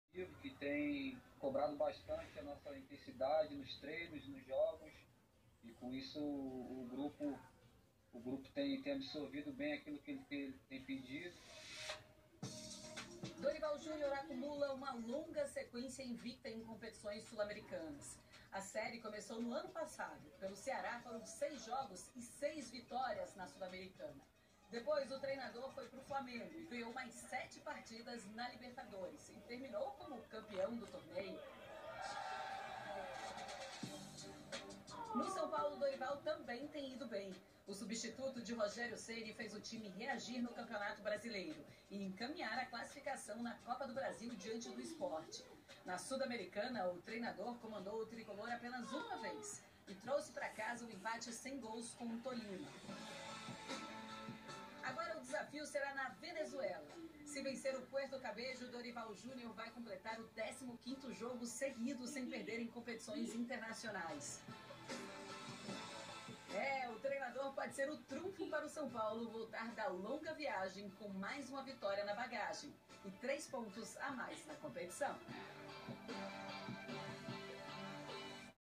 São Paulo na Venezuela joga contra Puerto Cabello pela Suldamericana
São Paulo FC joga na Venezuela contra o Puerto Cabello pela Suldamericana